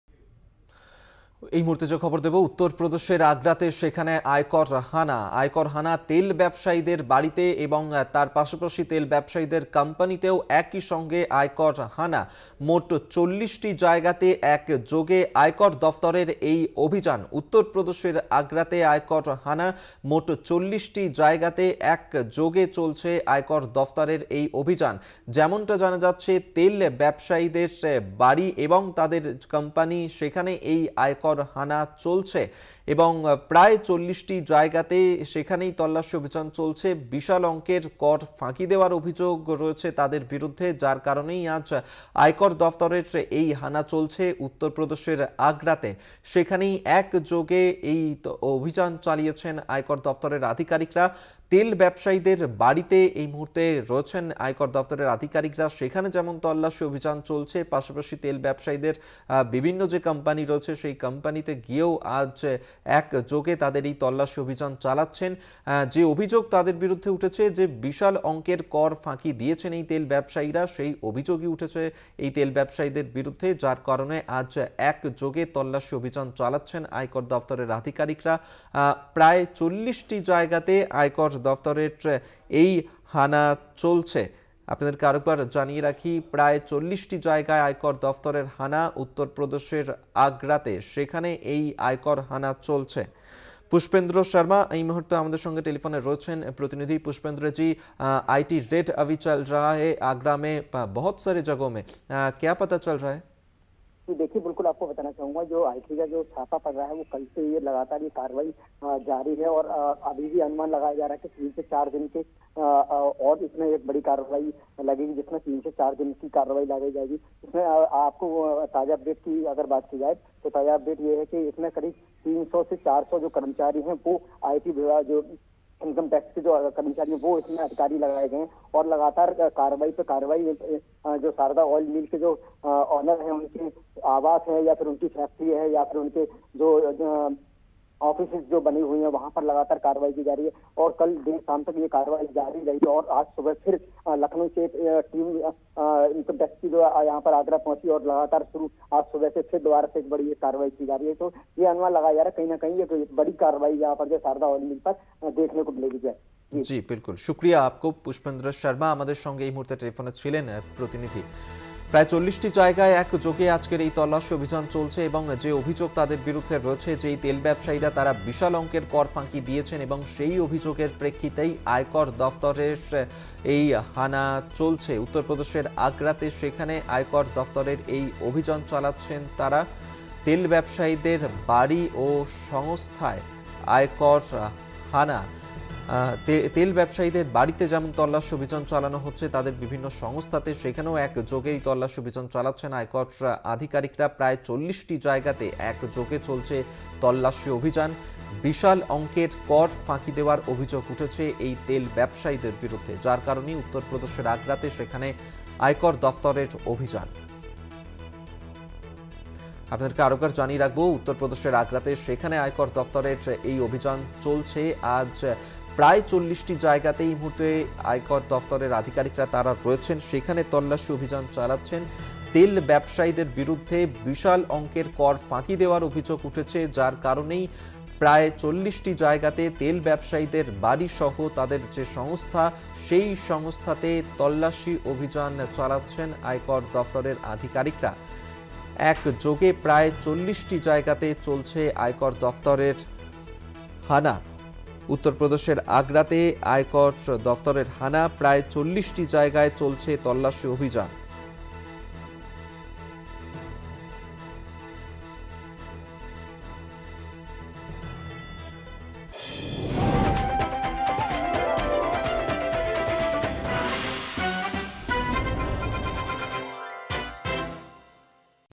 Uttar Pradesh | উত্তরপ্রদেশের তেল ব্যবসায়ীদের বাড়ি ও সংস্থায় আয়কর হানা, কর ফাঁকির অভিযোগ
website link for this copy :
@Kolkata Tv